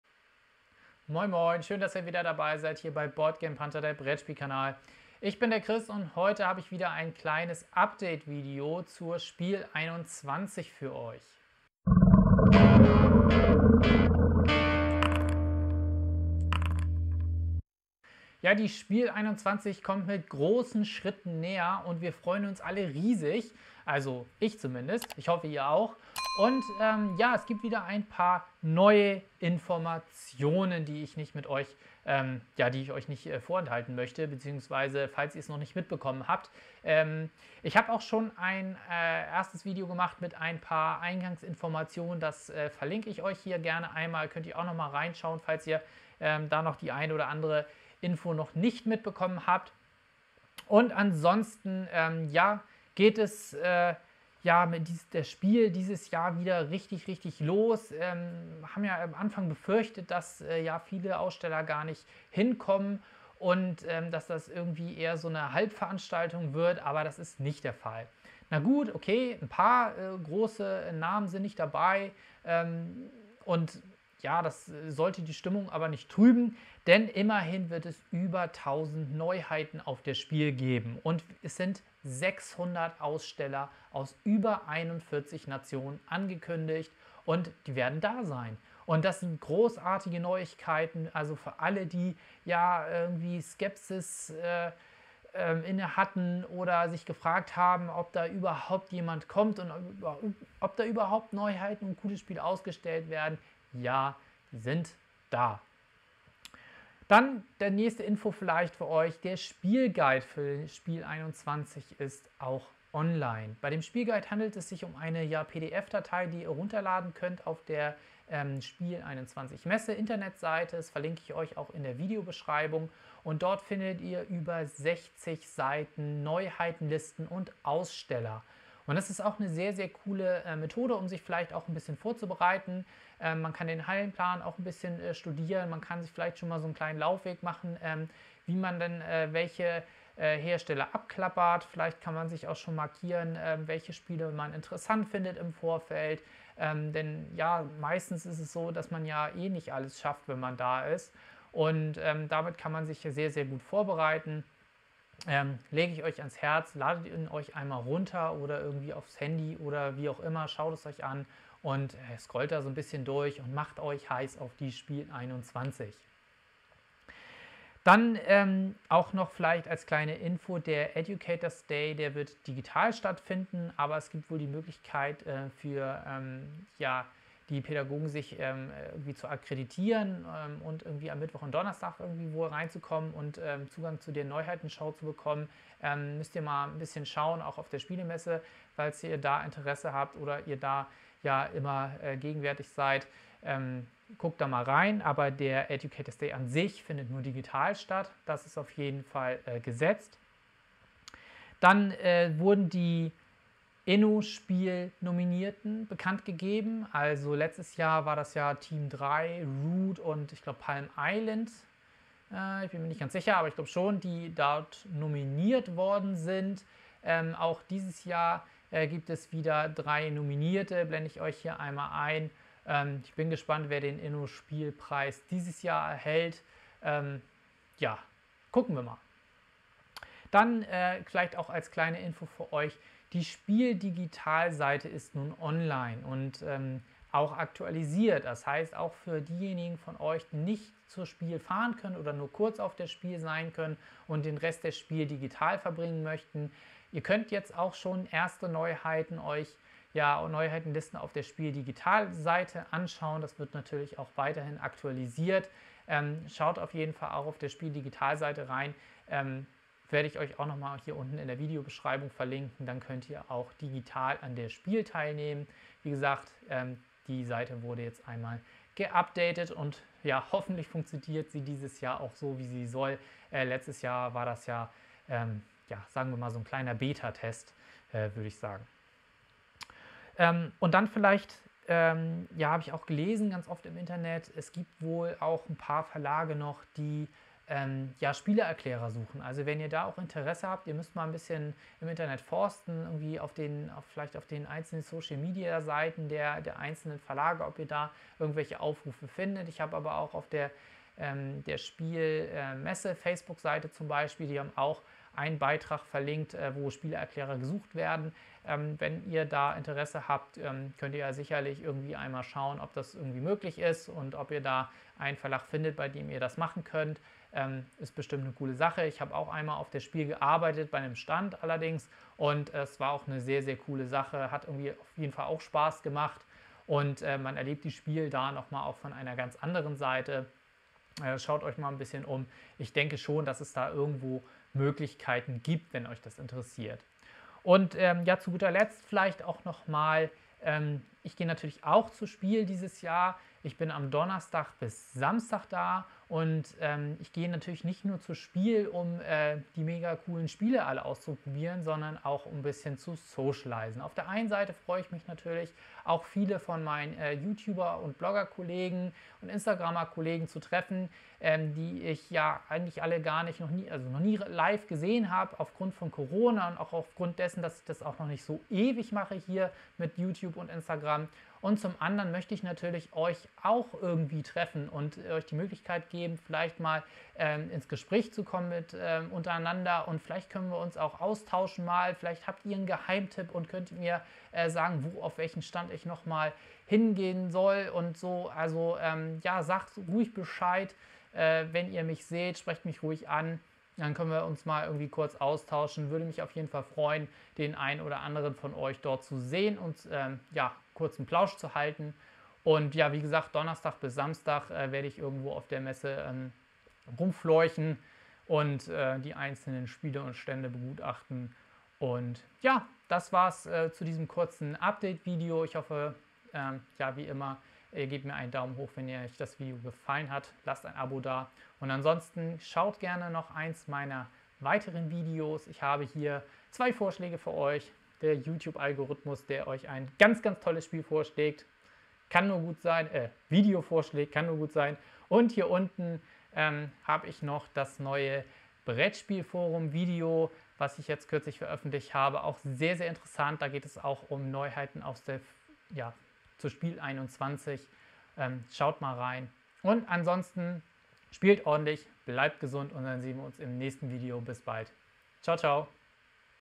Informationen zur Spiel '21 #2 - einige Neuigkeiten und wann ihr mich antreffen könnt!
Die Spiel '21 rückt näher und so langsam tauchen die ersten konkreten Informationen auf. Ich habe für euch diesem kurzen Video die aktuellen Infos einmal zusammengefasst und wenn ihr Lust auf einen kleinen Plausch habt, gibts auch die Info wann ihr mich auf der Spiel antreffen könnt.

🔔Wenn Du mich unterstützen willst dann freue ich mich über ein Abo von dir!  So verpasst du auch kein Video mehr 💗

🎲Unbezahlte Werbung! - Alle hier gezeigten Exemplare habe ich mir selbst gekauft